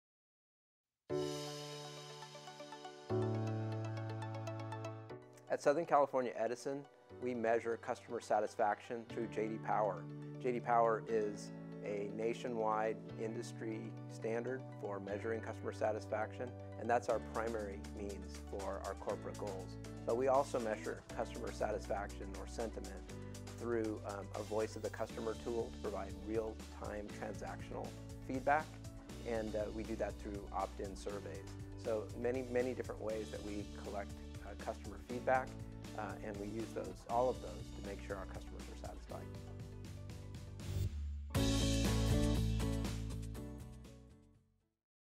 How Southern California Edison measures customer satisfaction
Stuart Hemphill, SVP, customer and operational services at Southern California Edison, explains how the utilities company measures customer satisfaction.

This interview is part of our Beyond Technology series, powered by ContactEngine.